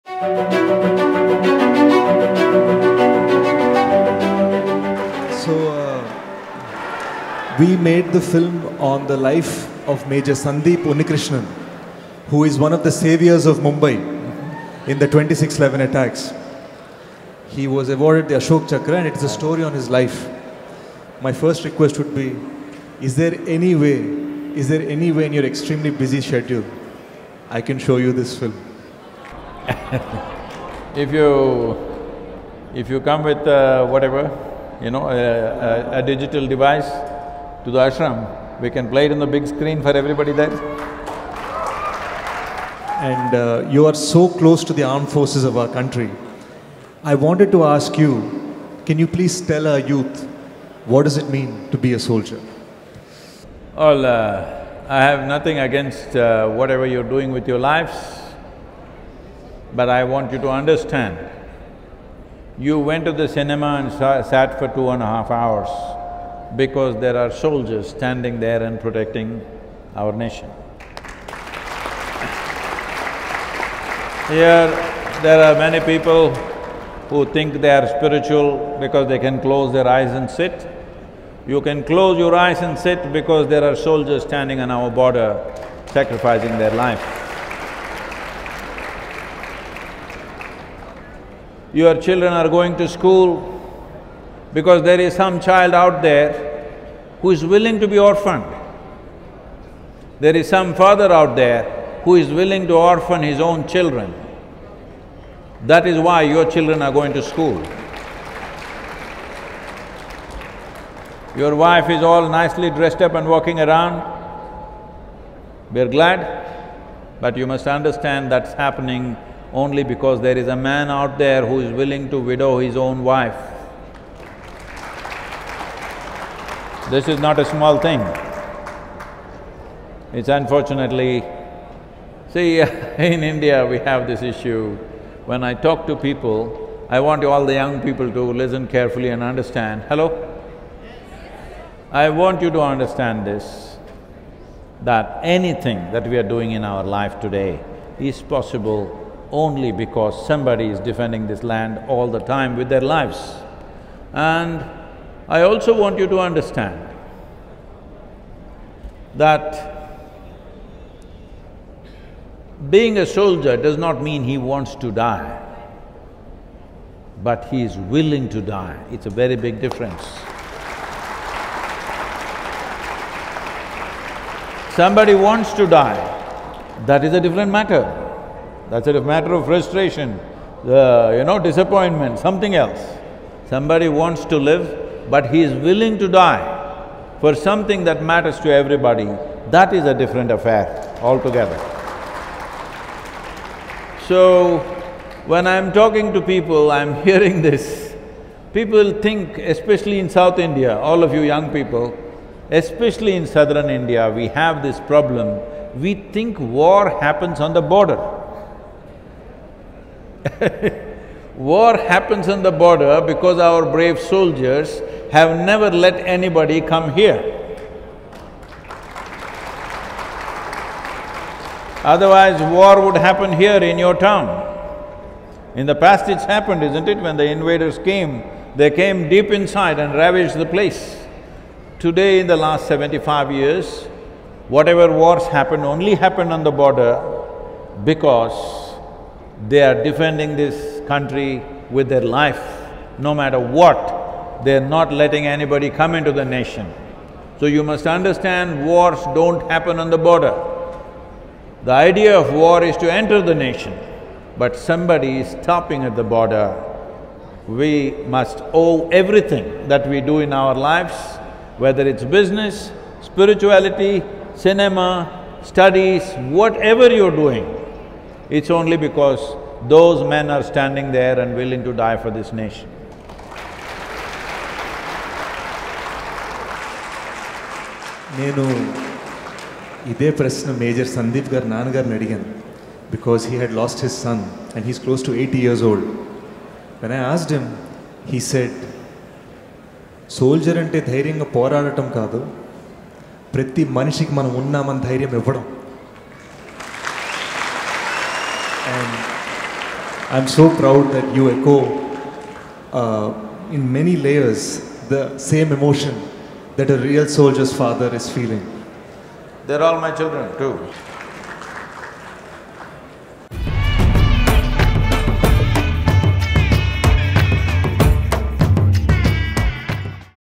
“What Does It Mean To Be A Soldier?” | Adivi Sesh Asks Sadhguru
Sadhguru explains what it means to be a soldier to actor Adivi Sesh at a Save Soil event in Kurnool, Andhra Pradesh.